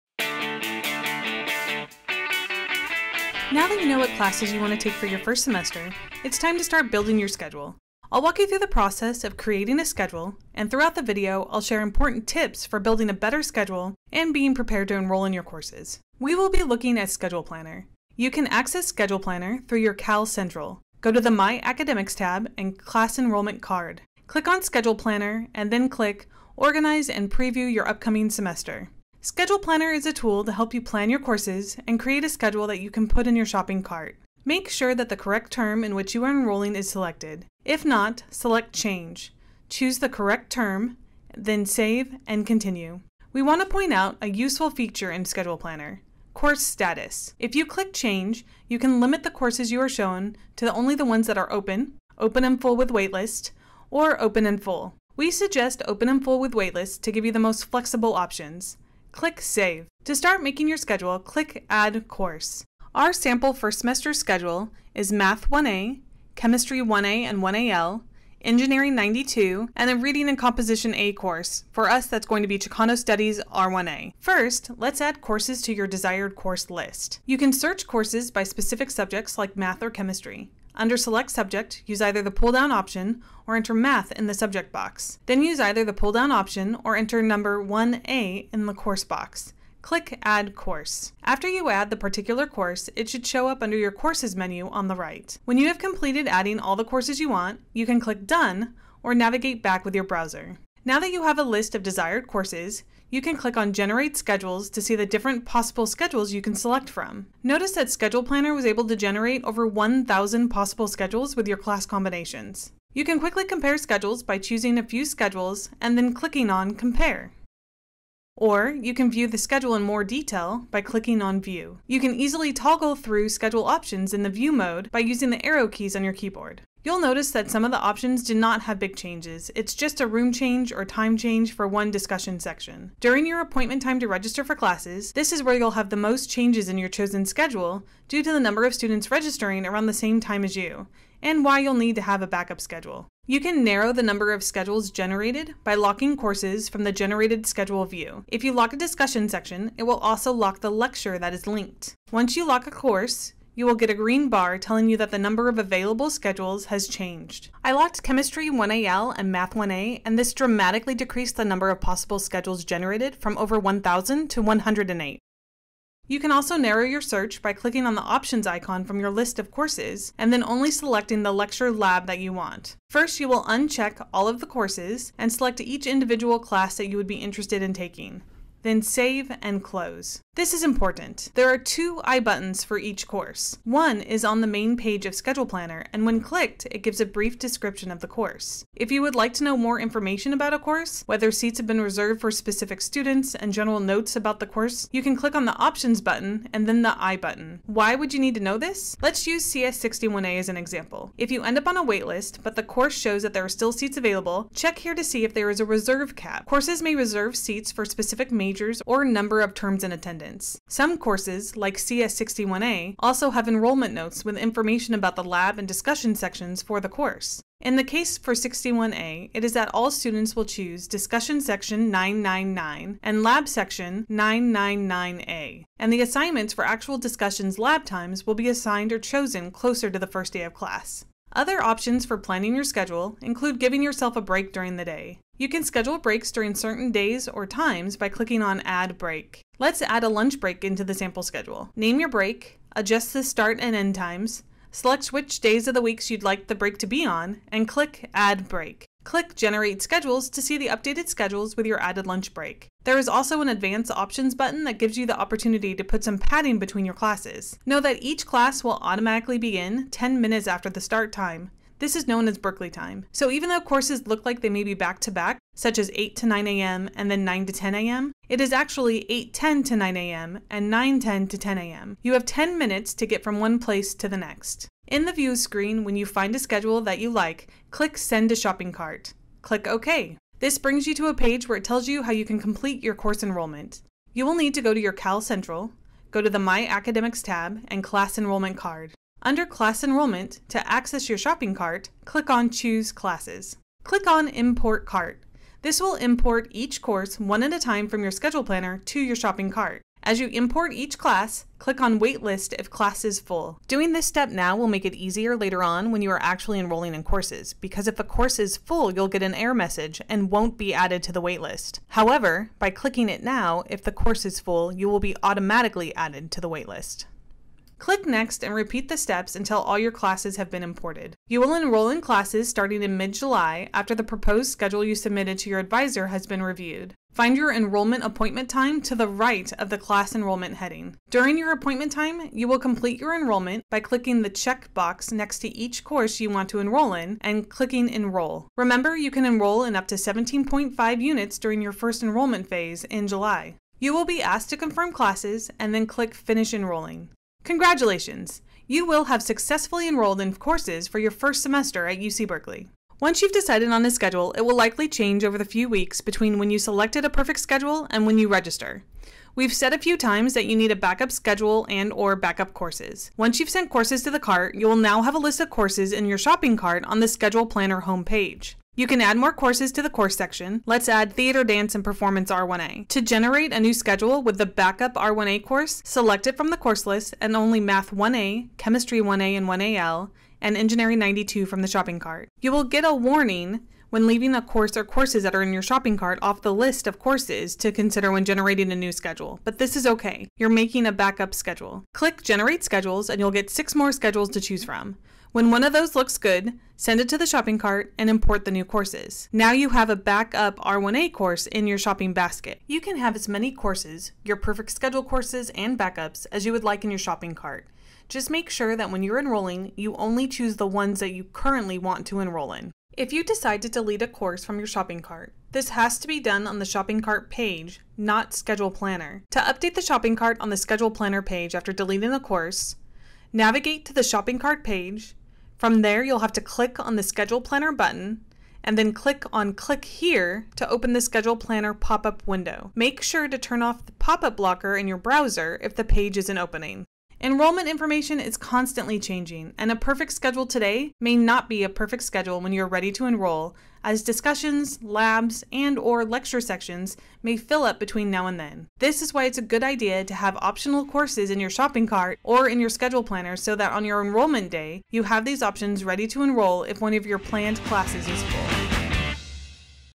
Schedule Planner Tips
UC Berkeley's Schedule Planner is available through CalCentral. It is where you will select courses, plan your schedule and send courses to you shopping cart.